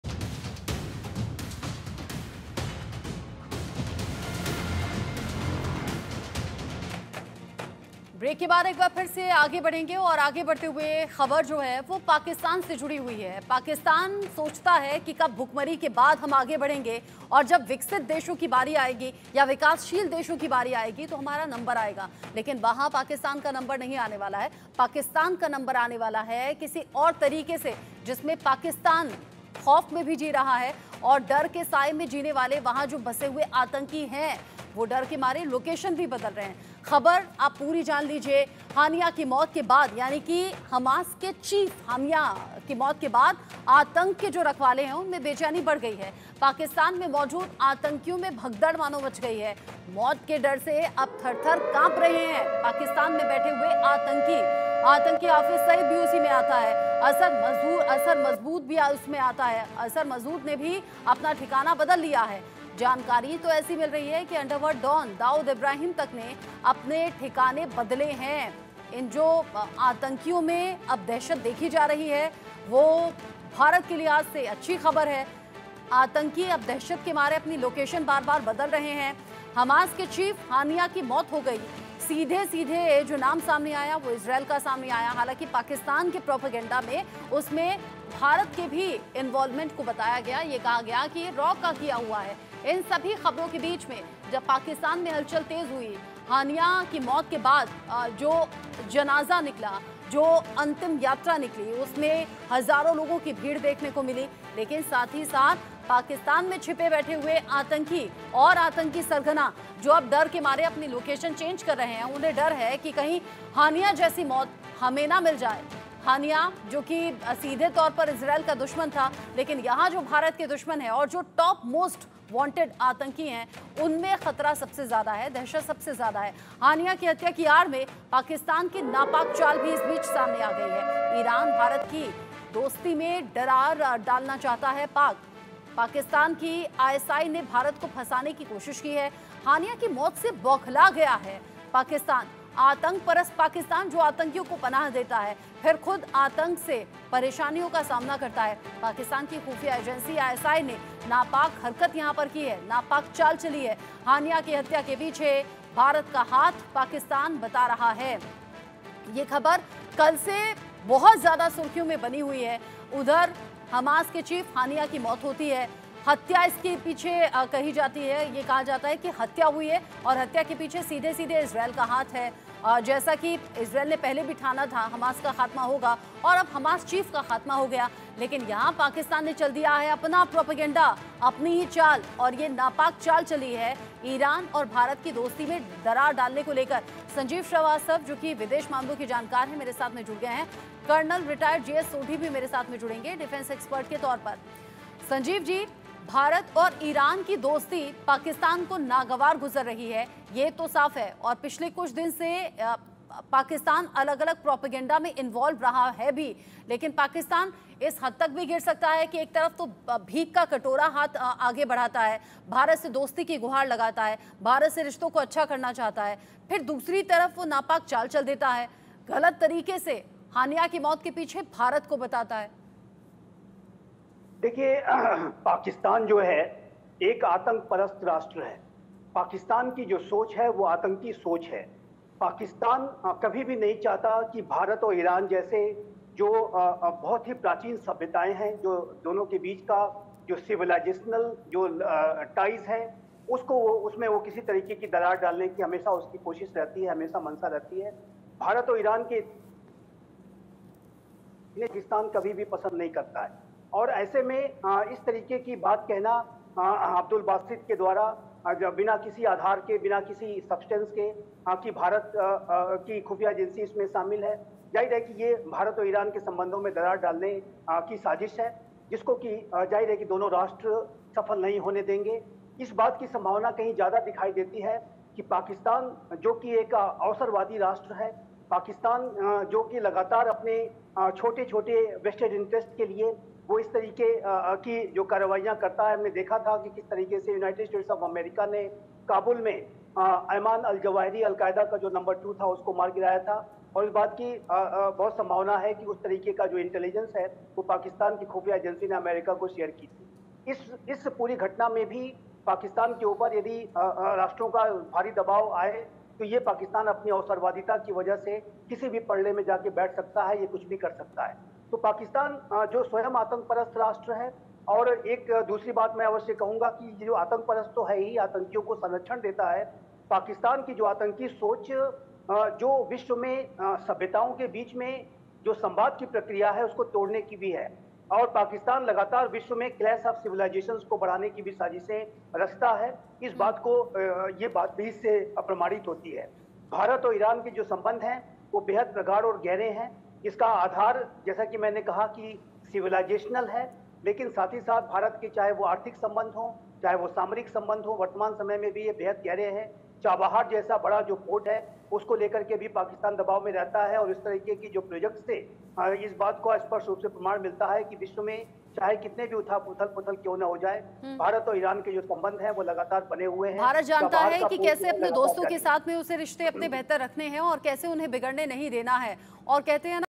Pakistan Breaking News: हानिया की हत्या पर पाकिस्तान की नापाक चाल | India News
हानिया की मौत से आतंक के रखवालो में बढ़ी बेचैनी बनी हुई है। पाकिस्तान में मौजूद आतंकियों में भगदड़ मच गयी है। मौत के डर से पाकिस्तानी आतंकी कांप रहे है। आतंकी हाफ़िज़ सईद और अज़हर मसूद ने अपना ठिकाना बदल दिया है। अंडर वर्ल्ड डॉन दाऊद इब्राहिम के खेमों में हलचल तेज़ हो गयी है।

There is increased restlessness among the guardians of terror due to Hania's death. There has been a stampede among the terrorists present in Pakistan. Pakistani terrorists are trembling due to fear of death. Terrorists Hafiz Saeed and Azhar Masood have changed their locations. The stir in the camps of underworld don Dawood Ibrahim has intensified.